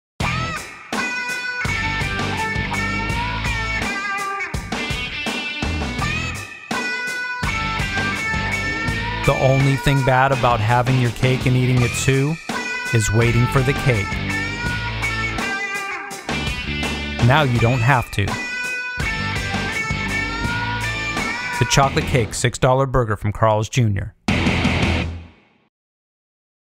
The Chocolate Cake Six Dollar Burger from Carl's Jr Parody
The only thing bad about having your cake and eating it too, is waiting for the cake.  Now you don't have to.

Written and Directed by:Matthew Solomon
DP: Eliseo Romero
Starring: AJ Almasi